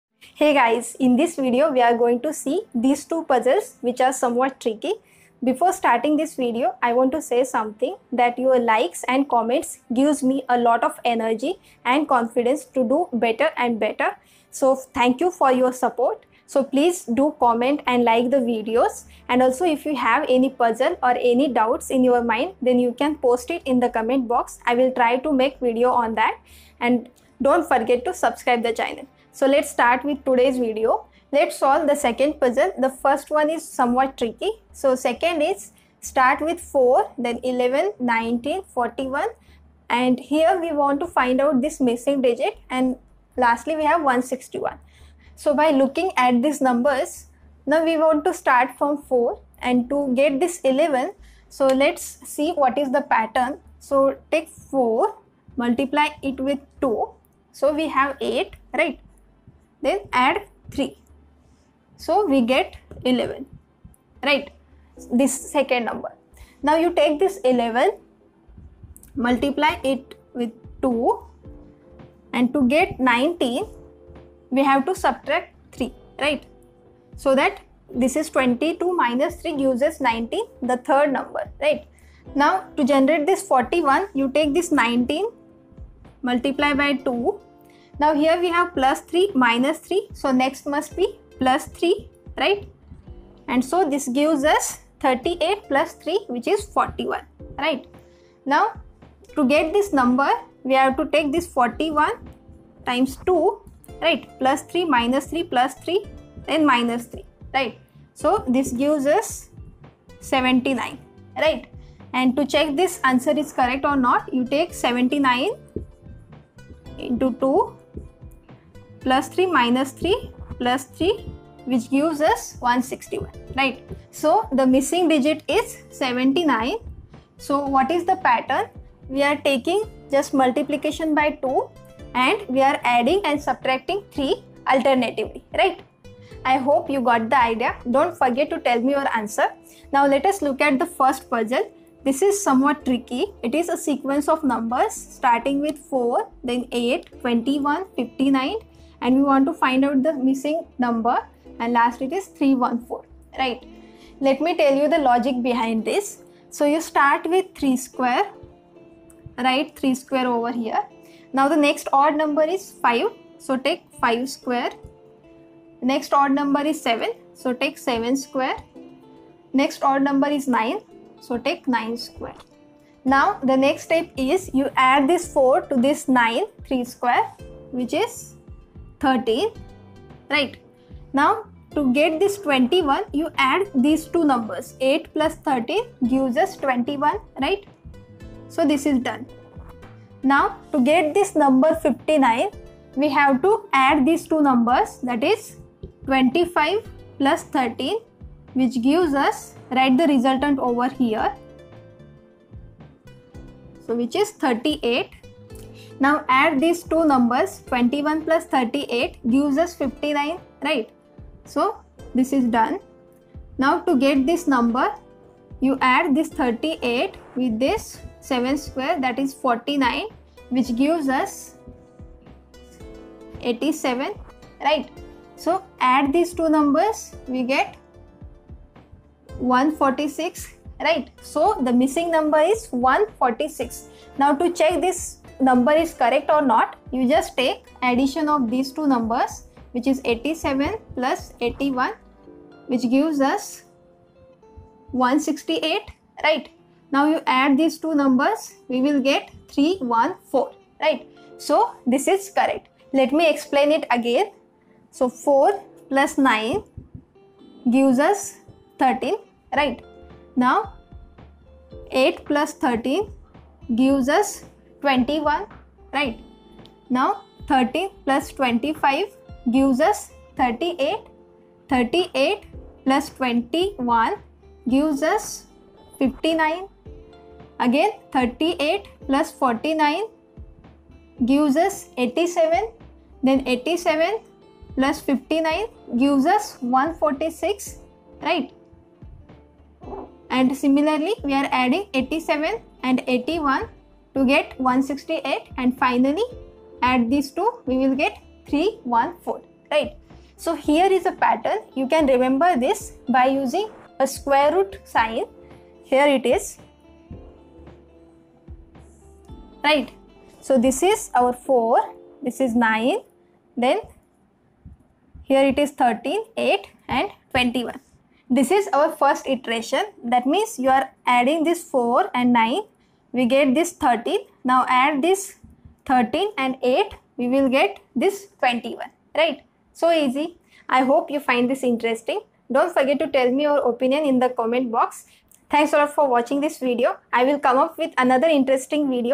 Can you find the missing number from these sequences? | Missing number puzzle
The most difficult puzzle. Can you find the missing number from these number sequences?  Can you solve this puzzle?  In this video I have solved a very interesting puzzle. In the following puzzles we have to find  a missing number from sequences.

1) 4, 11, 19, 41, ? , 161.
2) 4, 8, 21, 59, ? , 314...

3) Square root trick devi trick to calculate the cube of 2-digit number in 10 seconds | cube trick